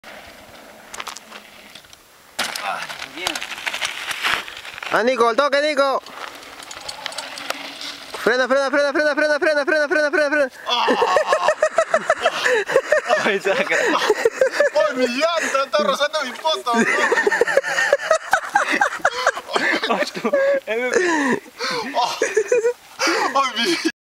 bajada cementos Lima
jeje aqui casi pierdo el control en esta bajada, rompi mi short nose una locura tremenda, nadie me dijo que tenia que frenar un poco antes de lanzarme jijij maldito ramon!!! jaja